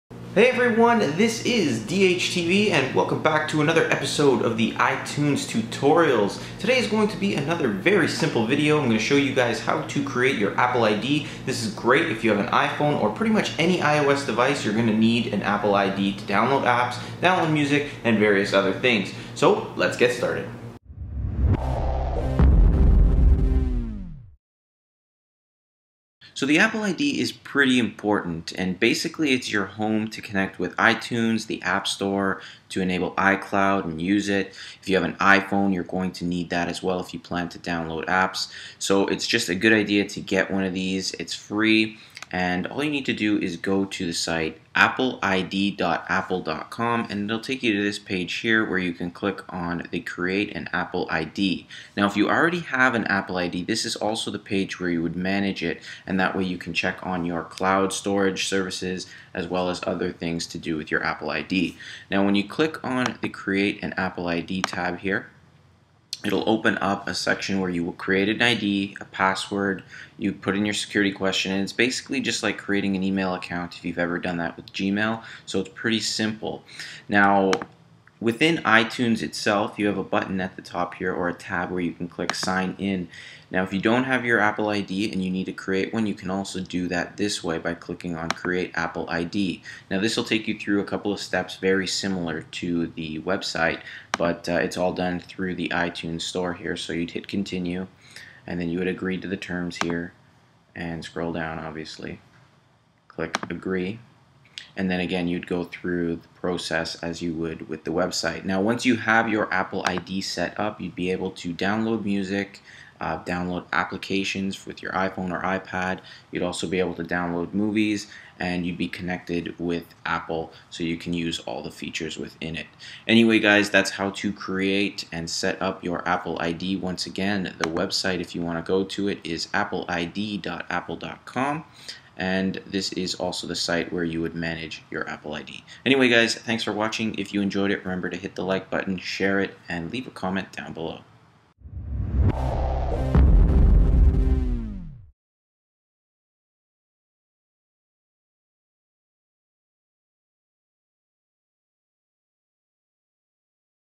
How To Setup and Create An Apple ID Account - iTunes Tutorials
Full tutorial on how to setup and create an Apple ID account to use with iTunes, the app store and any iOS device.